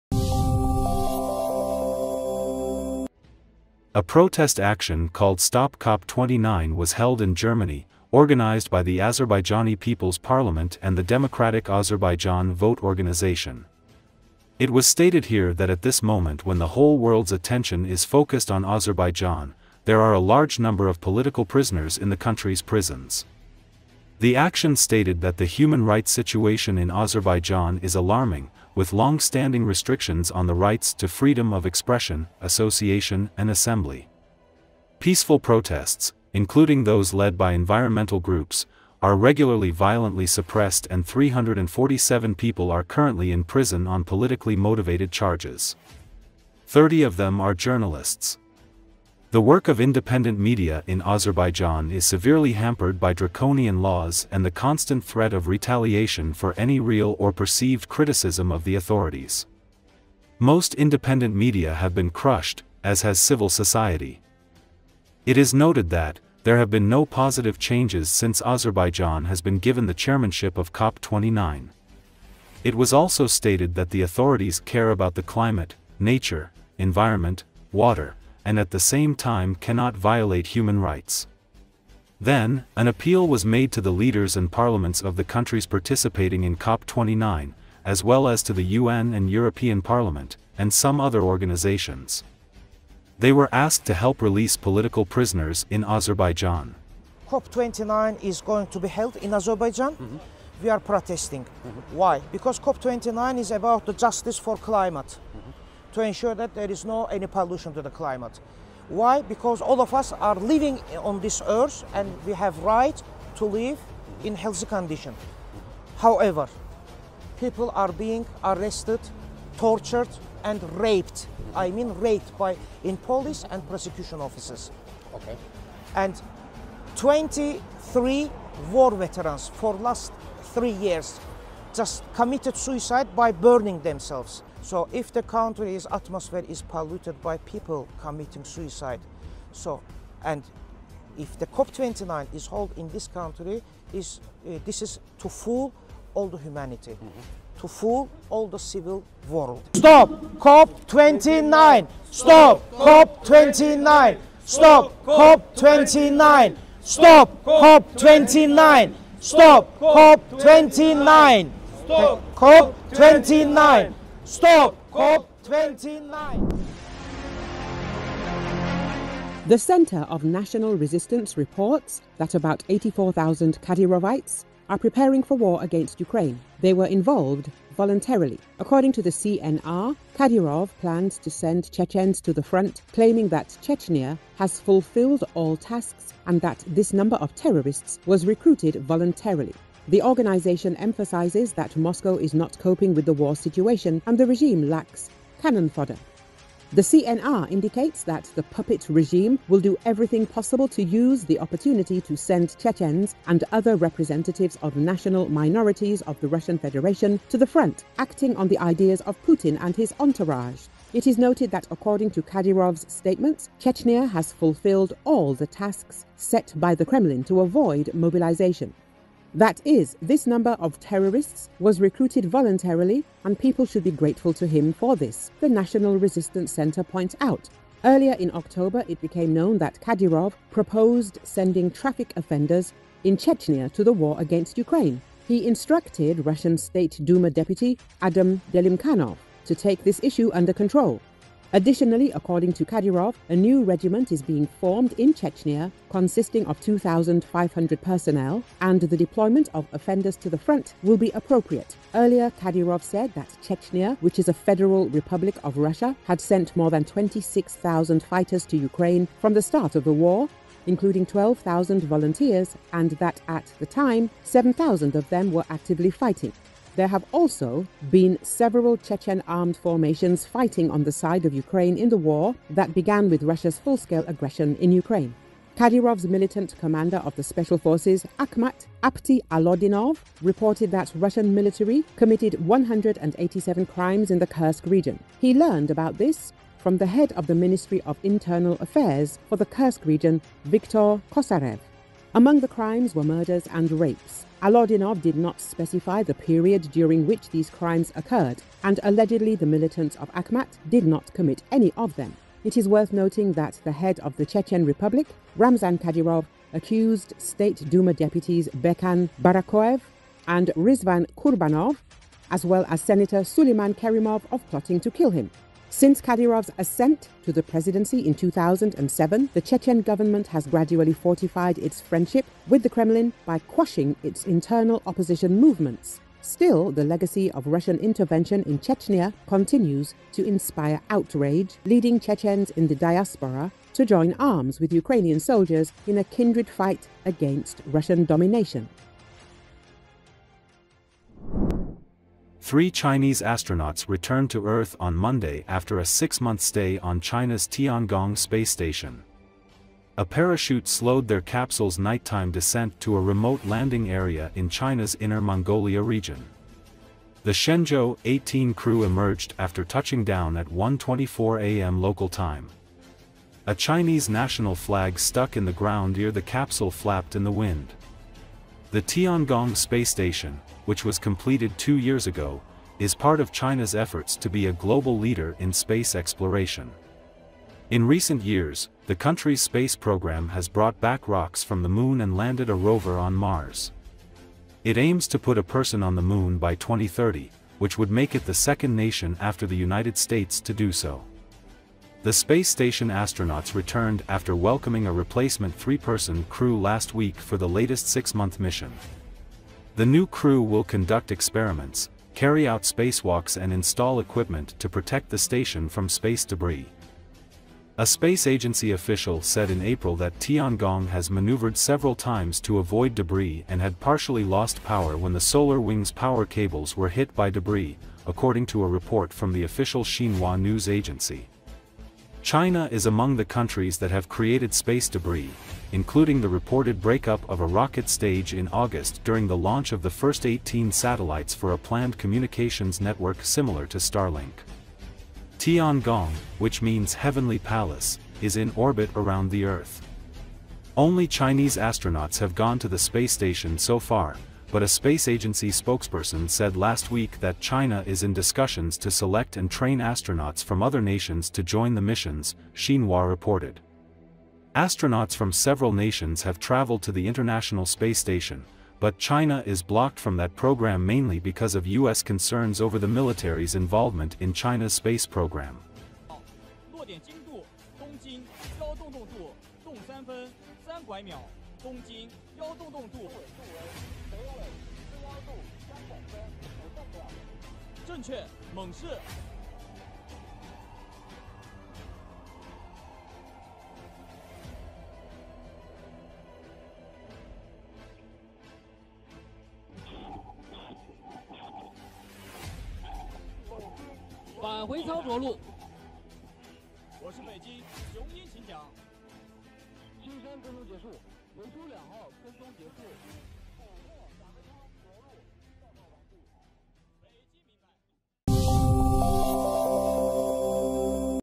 Azerbaijani political emigrants held STOP COP29 protest action in Germany
A protest action called STOP COP29 was held in Germany, organized by the Azerbaijani People's Parliament and the Democratic Azerbaijan Vote organization. It was stated here that at this moment when the whole world's attention is focused on Azerbaijan, there are a large number of political prisoners in the country's prisons.
The action stated that the human rights situation in Azerbaijan is alarming, with long-standing restrictions on the rights to freedom of expression, association and assembly. Peaceful protests, including those led by environmental groups, are regularly violently suppressed and 347 people are currently in prison on politically motivated charges. 30 of them are journalists.
The work of independent media in Azerbaijan is severely hampered by draconian laws and the constant threat of retaliation for any real or perceived criticism of the authorities. Most independent media have been crushed, as has civil society.
It is noted that, there have been no positive changes since Azerbaijan has been given the chairmanship of COP29.
It was also stated that the authorities care about the climate, nature, environment, water, and at the same time cannot violate human rights.
Then, an appeal was made to the leaders and parliaments of the countries participating in COP29, as well as to the UN and European Parliament, and some other organizations. They were asked to help release political prisoners in Azerbaijan.